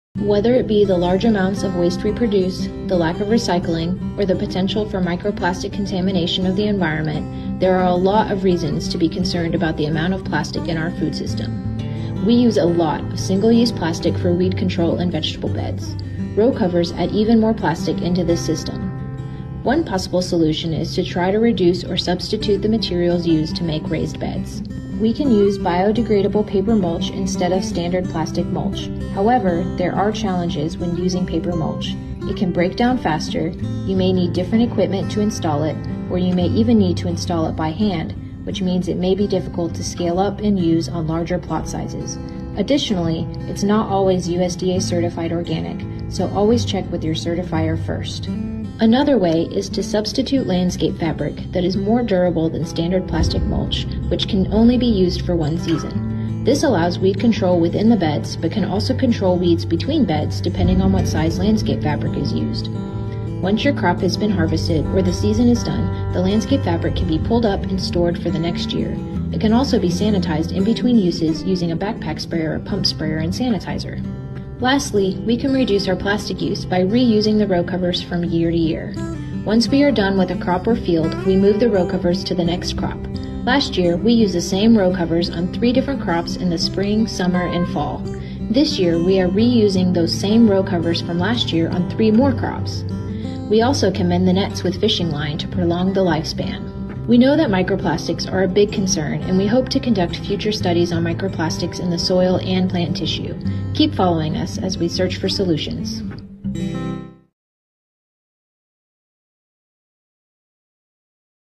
The Plastic Dilemma in Agriculture
Agriculture contributes to just over 3% of global plastic waste, but that percentage still represents a substantial and growing input. Alongside plasticulture, tools like mesotunnels introduce additional layers of plastic into production. Join us as we explore the role of plastic in organic systems and ask a critical question: Can plastic ever align with the principles of sustainability? Here's what the data and our experience suggests so far.